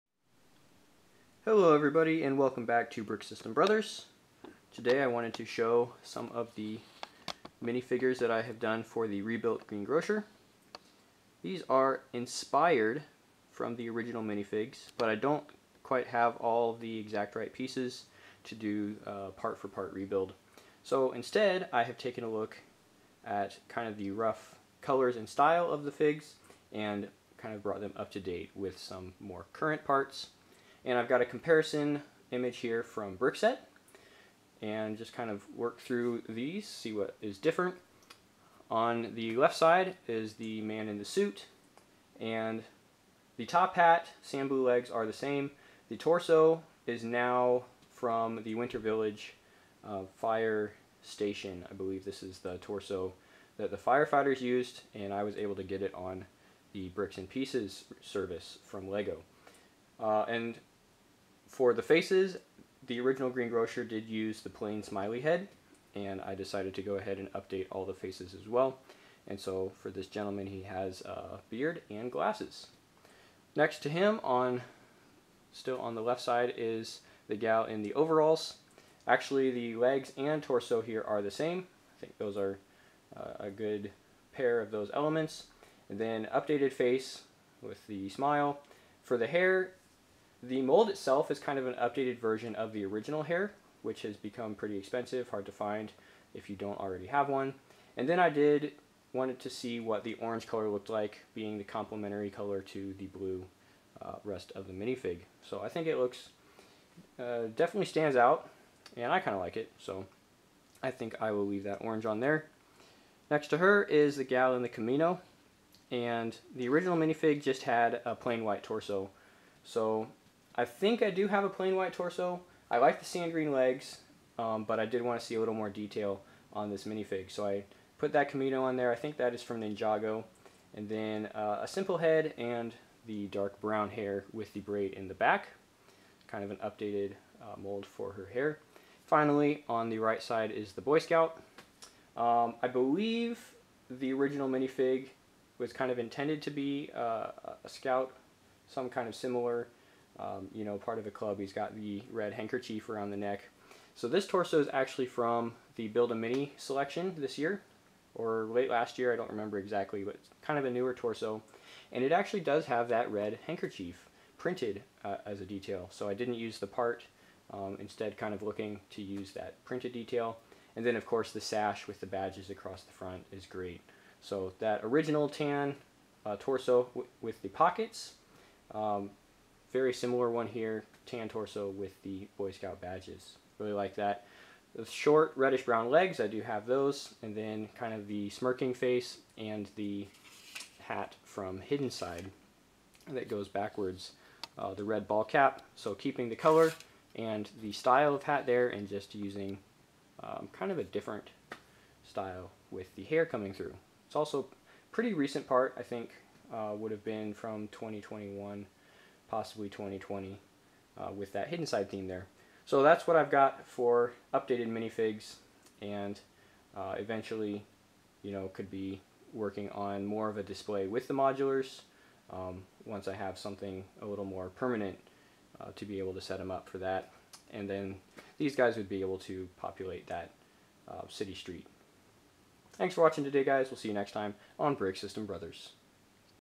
Modern Minifigures for Green Grocer
Even as I have made minor adjustments to the modular building itself, I wanted to see what kind of changes I could bring to the Minifigure side of 10185.